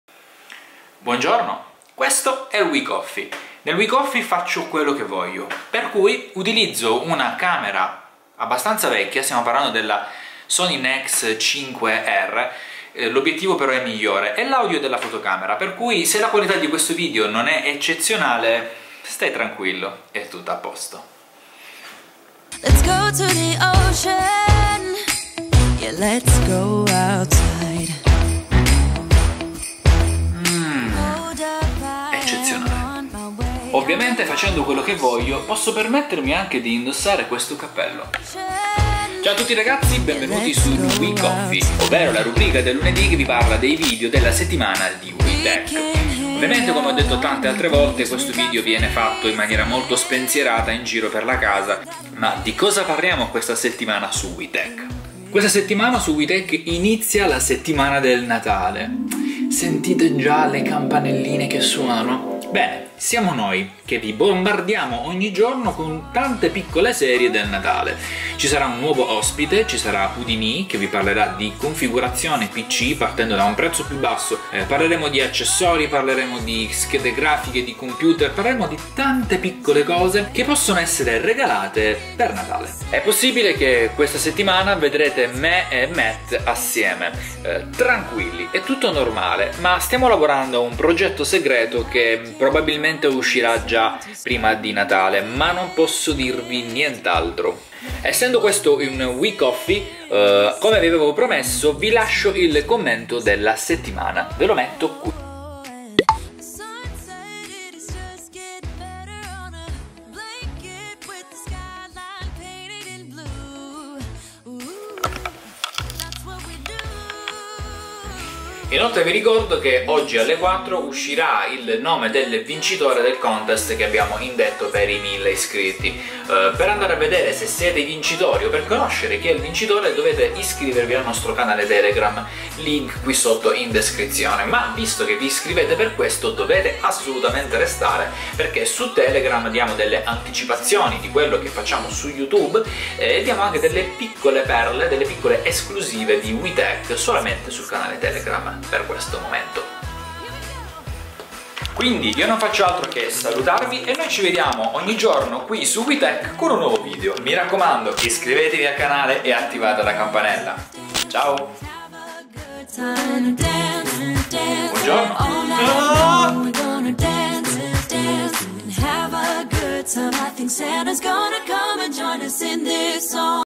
REGALI DI NATALE, siete pronti?
Questo è il wee_tech, il video del lunedi che da le news, le anticipazoini della settimana e tanto altro qui su wee_tech.

Questa settimana REGALI DI NATALE TECH